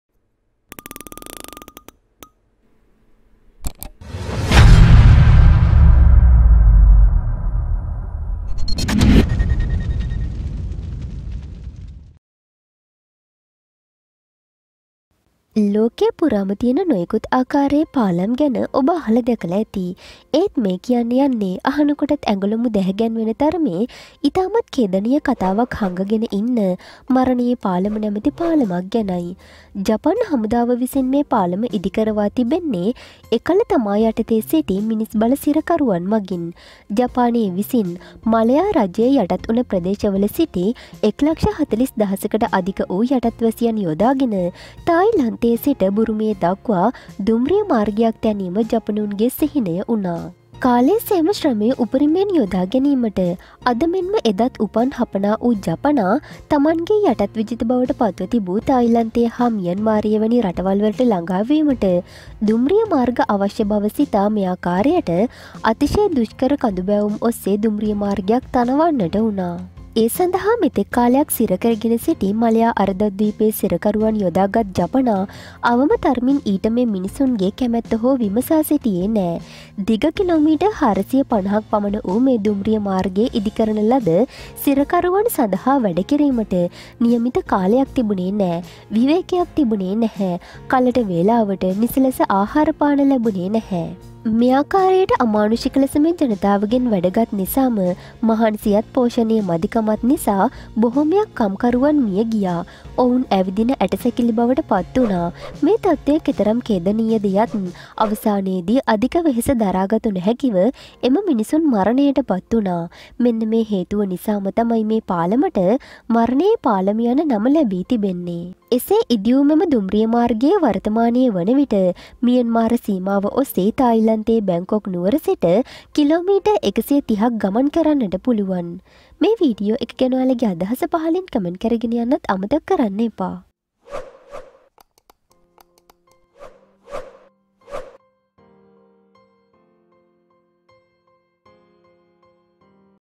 ලොවක් කම්පා කළ පාලම අදටත් සංවේදි - Myanmar - Thai train bridge
ලෝකය පුරාම තියෙන නොයෙකුත් ආකාරයේ පාලම් ගැන ඔබ අහලා දැකලා ඇති. ඒත් මේ කියන්න යන්නේ අහනකොටත් ඇඟ ලොමු දැහැගැන්වෙන තරමේ ඉතාමත් ඛේදනීය කතාවක් හංගගෙන ඉන්න, මරණයේ පාලම නමැති පාලමක් ගැනයි